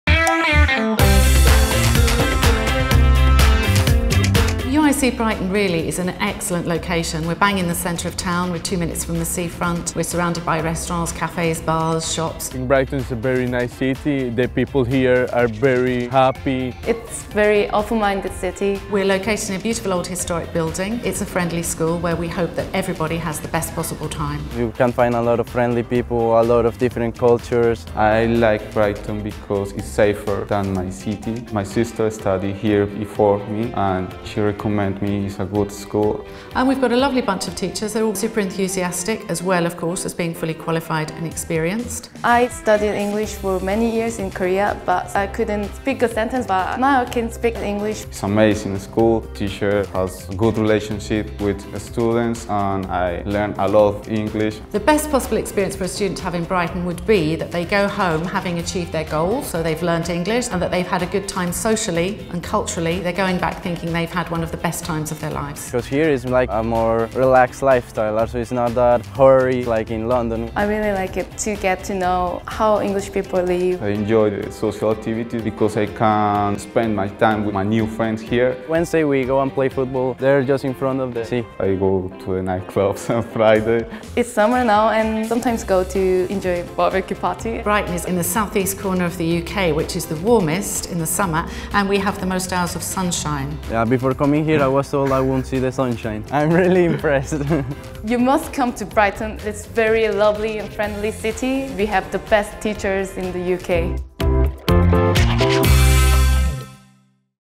Oxford International Education Group | English Brighton 2015
Our School of English in Brighton may have everything you’re looking for. Brighton is a vibrant seaside town, famous for both its heritage and very modern bohemian, arty lifestyle. When you arrive at our Brighton school, you will join a small and friendly community of students and staff who will encourage you to reach the highest level in your English language skills.